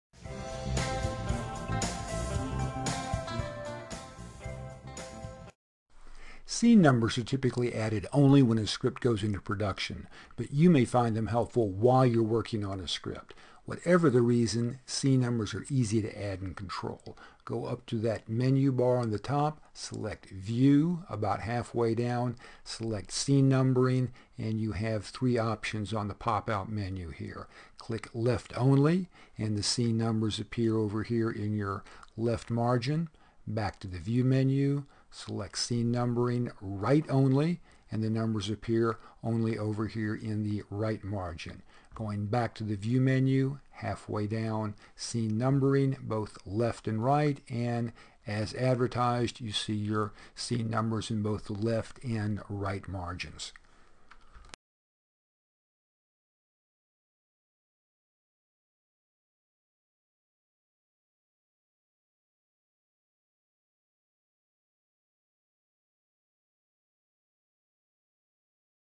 Controlling Elements: Number Your Scenes 5/5
In this lesson, you will learn how to number your scenes in Movie Magic Screenwriter.